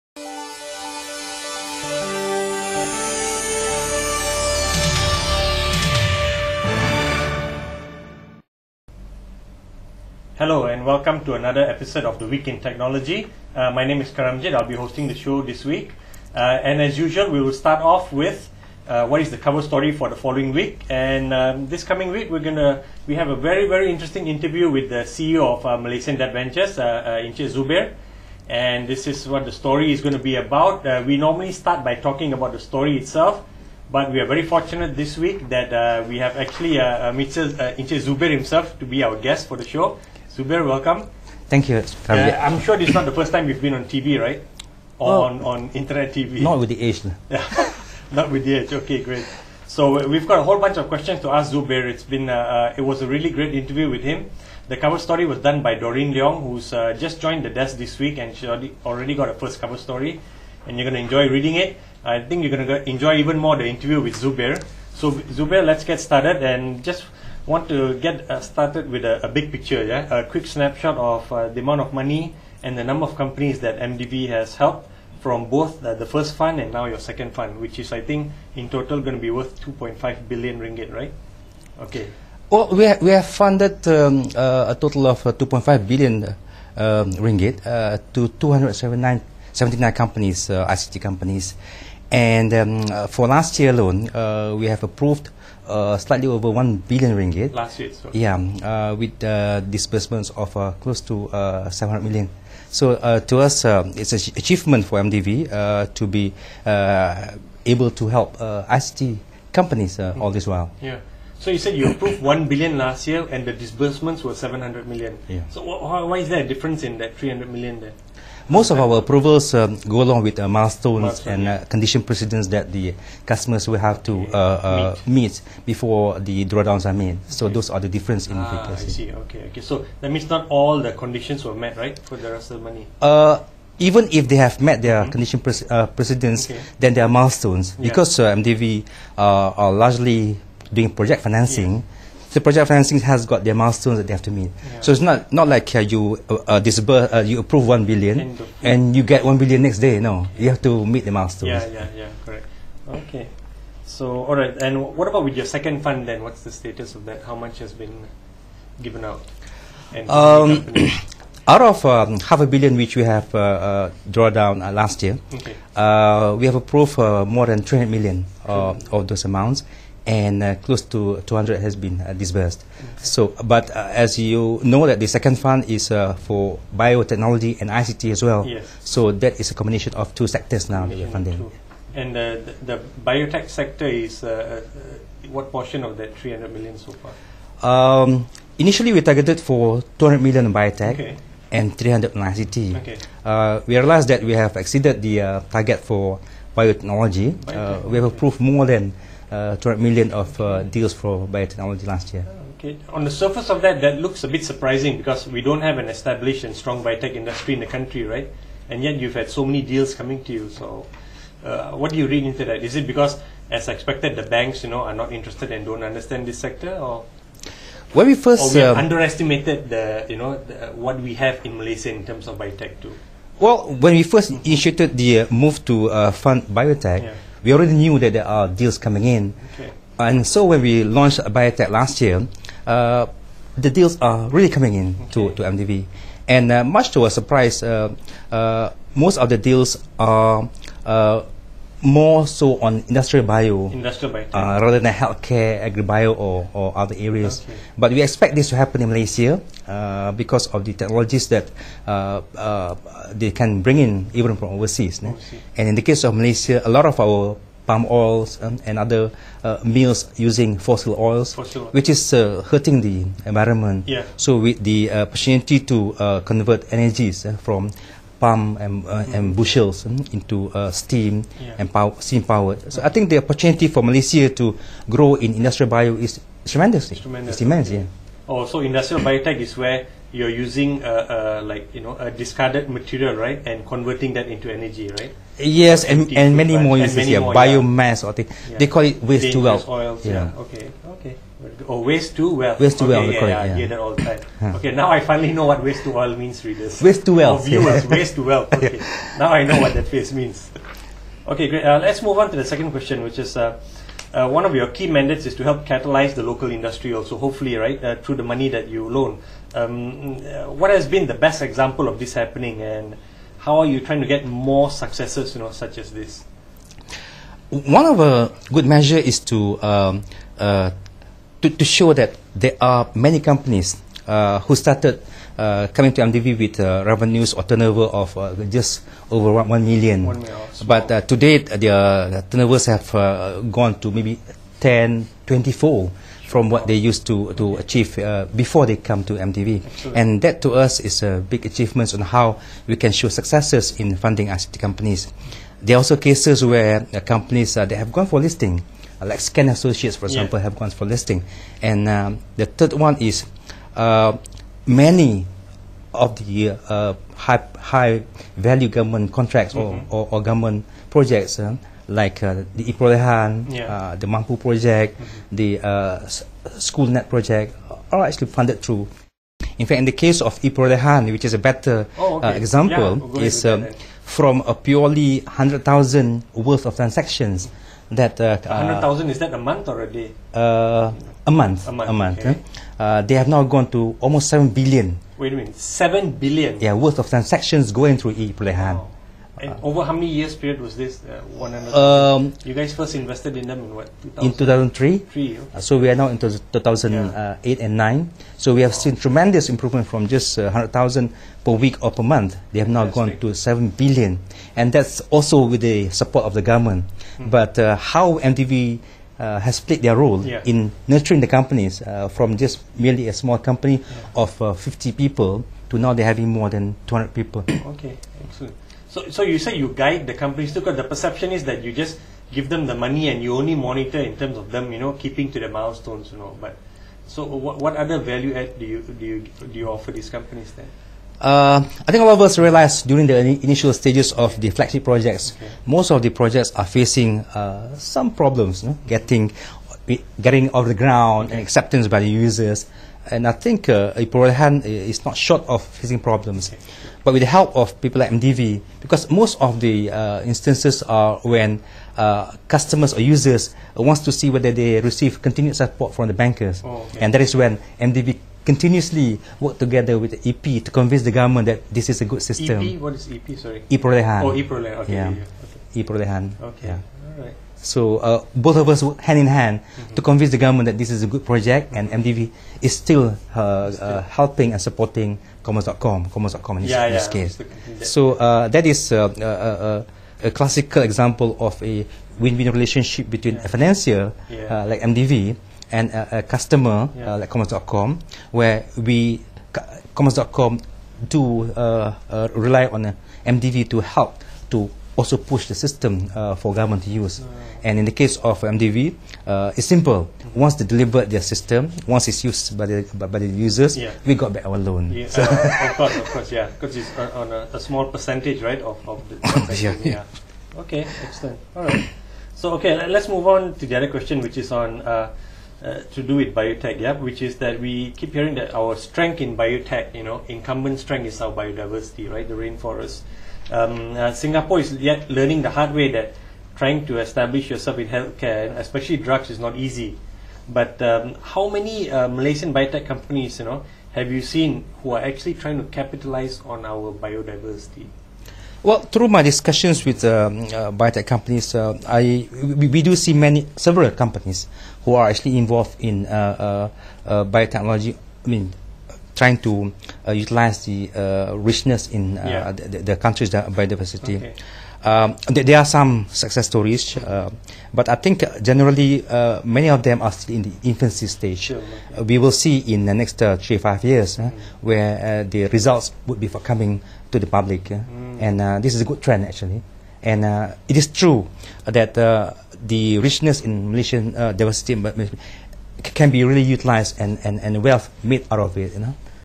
Week In Tech - MDV CEO Zubir Ansori Yahya (Part 1)
Week in Tech speaks to MDV CEO Zubir Ansori Yahya about their new biotech fund, a gap in the funding ecosystem and how a company they helped went from RM100,000 transaction online a month to RM7 billion !